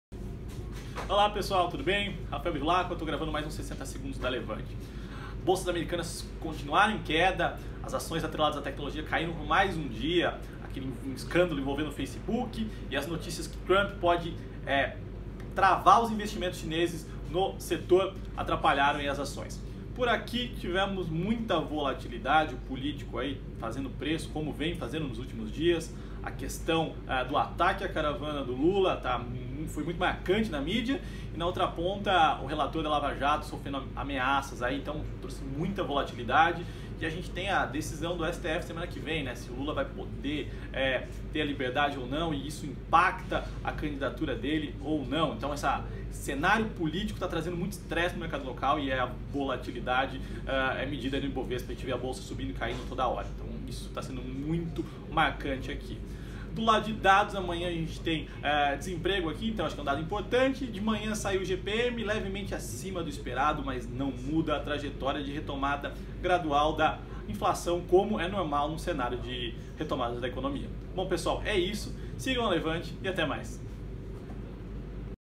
60 segundos Levante - As notícias que vão mexer com o seu bolso em ​28/03
Volatilidade na Bolsa brasileira​. Assista ao 60 Segundos com ​Rafael Bevilacqua

Quer receber os nossos conteúdos gratuitos no seu e-mail? Cadastre-se em levanteideias.com.br